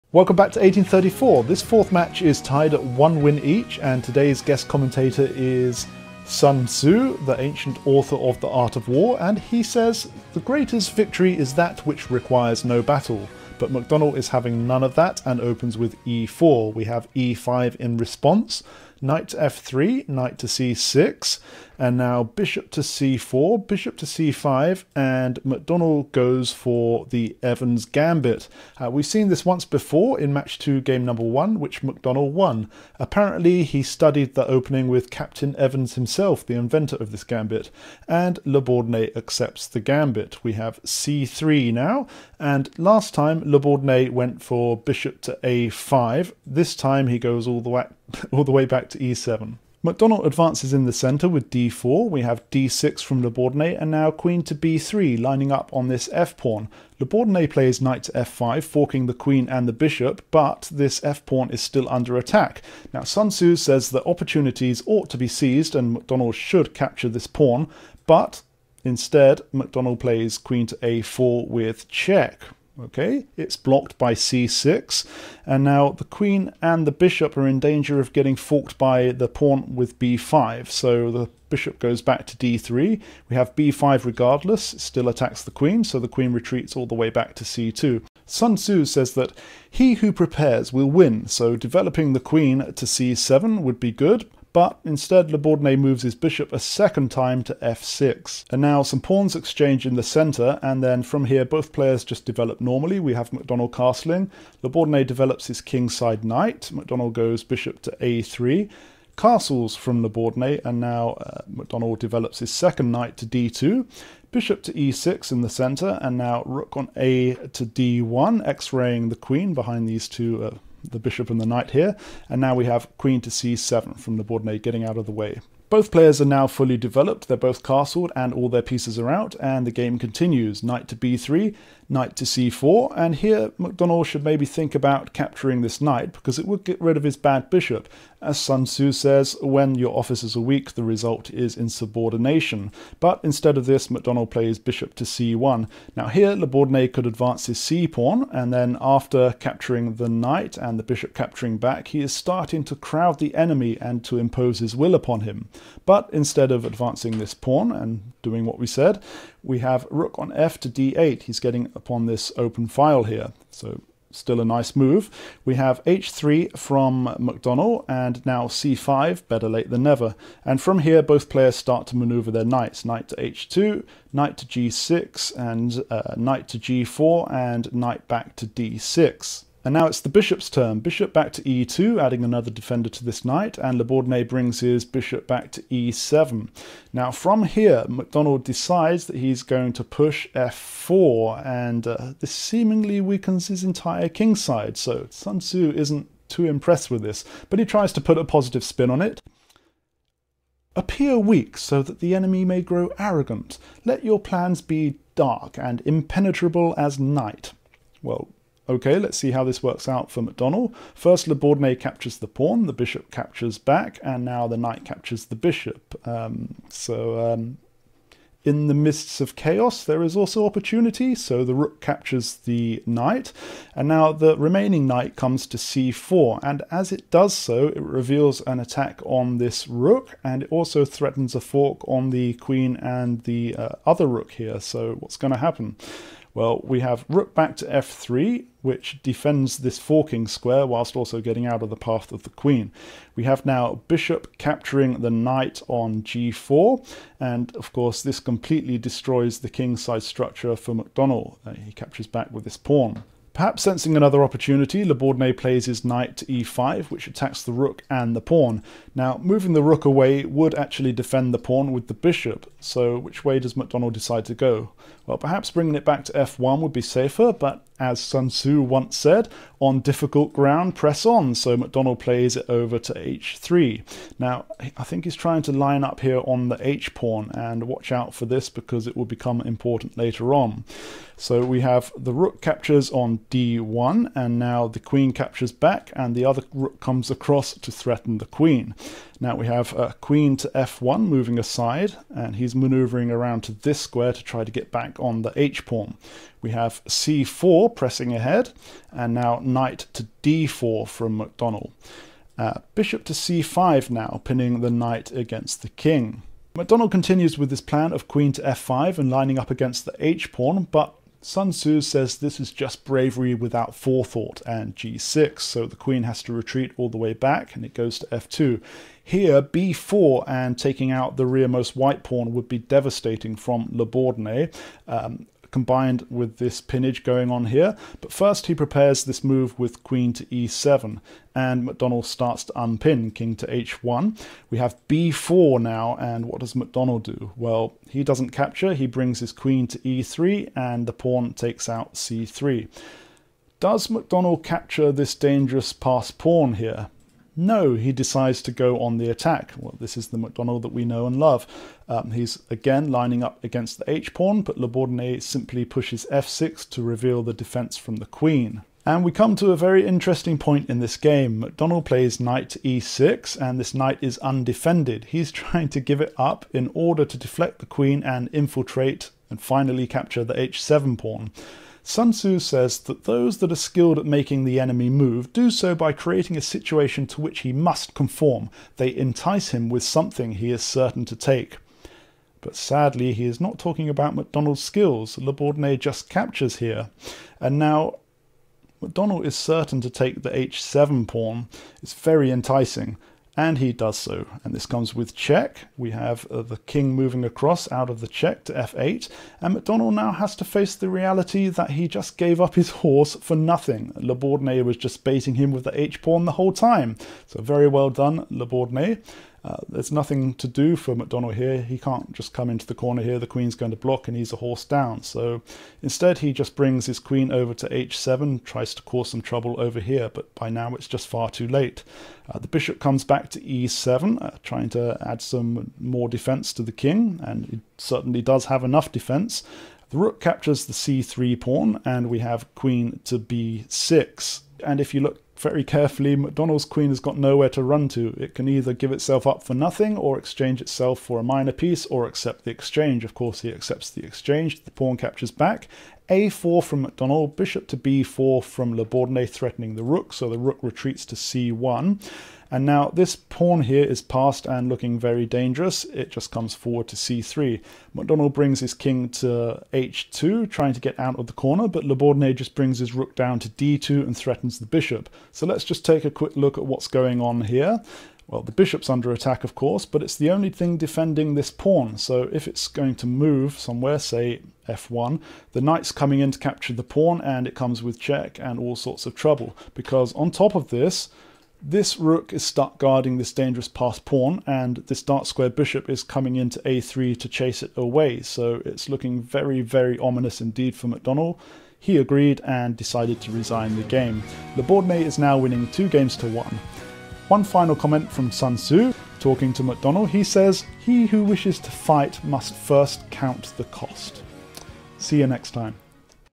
ALL warfare is based on DECEPTION! 1834 World Chess Championship [Match 4, Game 5]
This is the fifth game of the fourth 1834 unofficial world chess championship match between Louis-Charles Mahé de La Bourdonnais and Alexander McDonnell.

For this game, we draw upon the ancient wisdom of Sun Tzu, the Chinese military general, strategist, philosopher, and writer who lived during the Eastern Zhou period of 771 to 256 BCE.

Will McDonnell demonstrate honour, courage and wisdom and emerge victorious?
Will La Bourdonnais show McDonnell the true way of the warrior?
There can only be one winner!